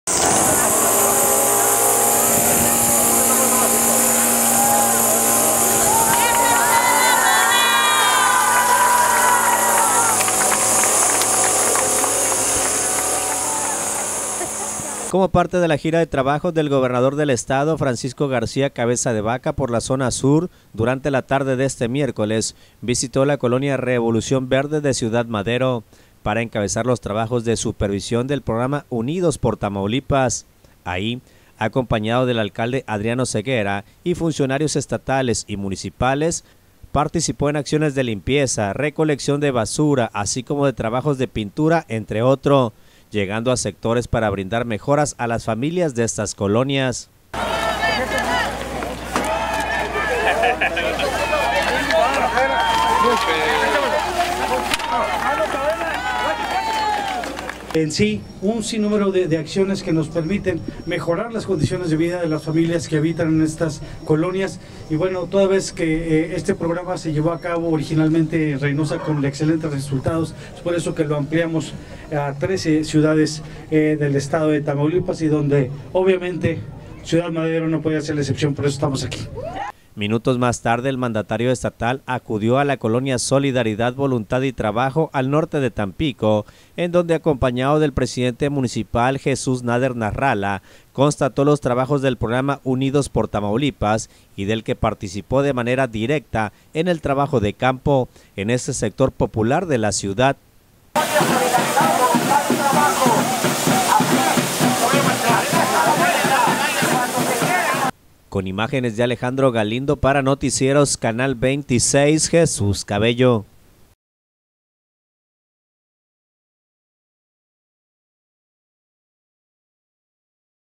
GOBERNADOR EN CABEZA "UNIDOS POR TAMAULIPAS".
Como parte de la gira de Trabajo del gobernador del estado Francisco García Cabeza de Vaca por la zona sur durante la tarde de este miércoles, visitó la Col. Revolución Verde de ciudad Madero para encabezar los trabajos de supervisión del programa “Unidos por Tamaulipas”.